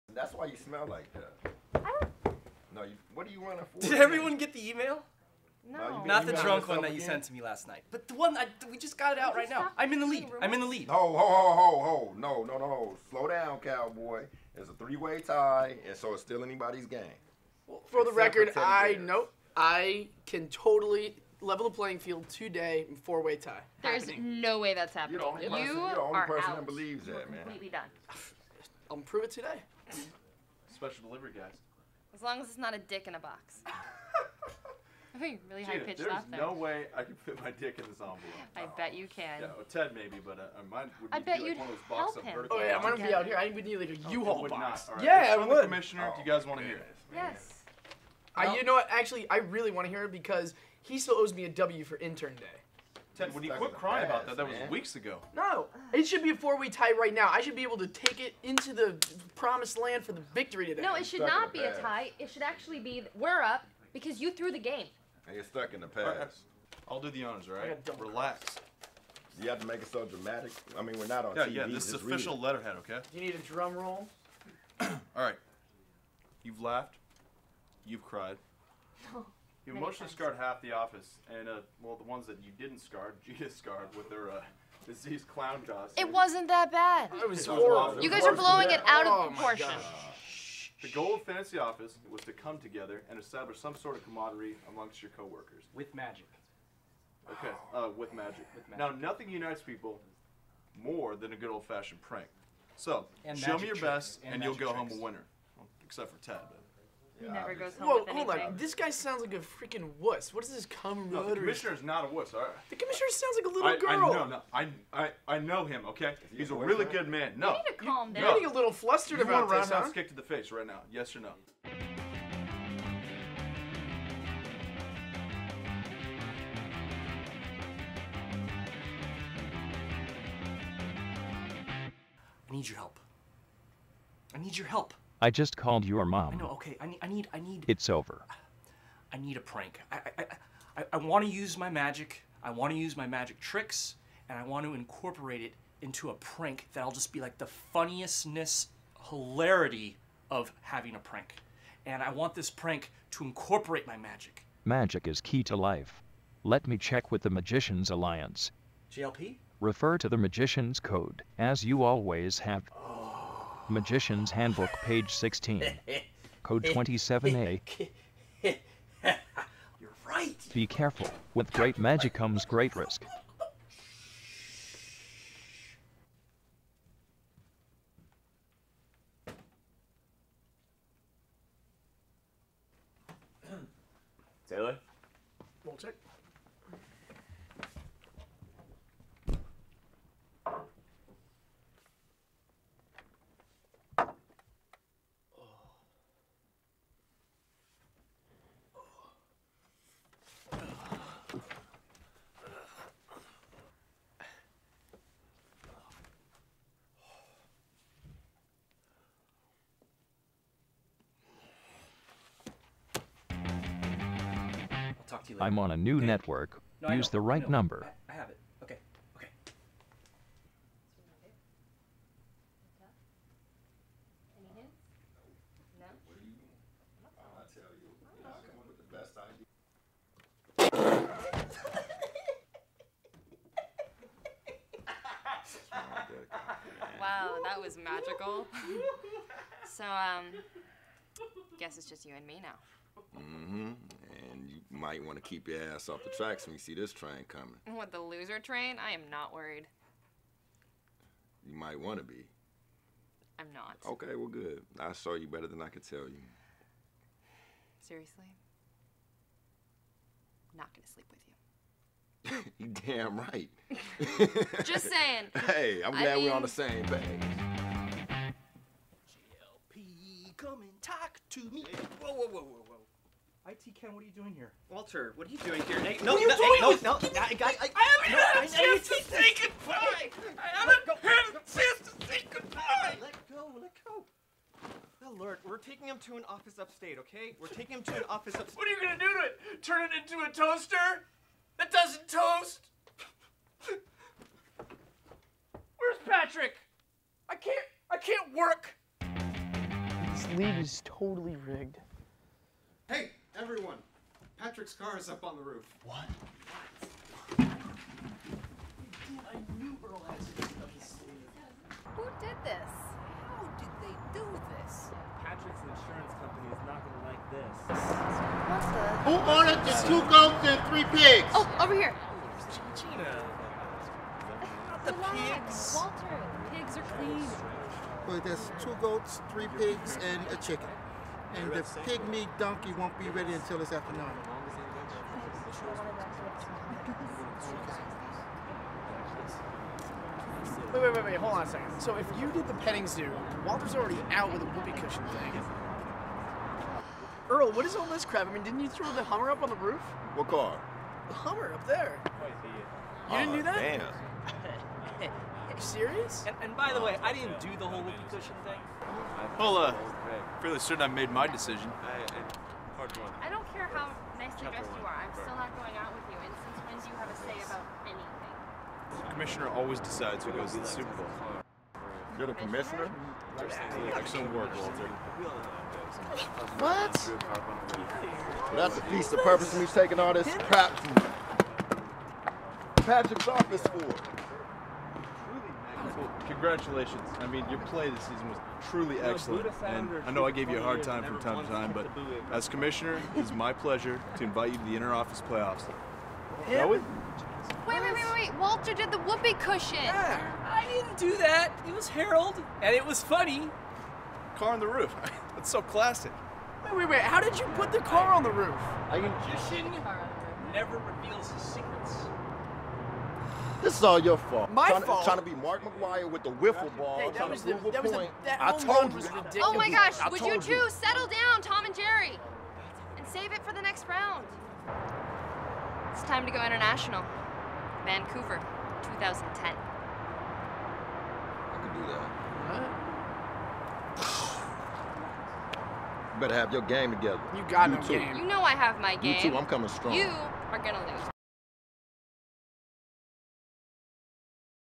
Fantasy Office Week 16 "Superbowl"
This is it! This is what we've all been waiting for. Find out who wins the first ever Fantasy Office!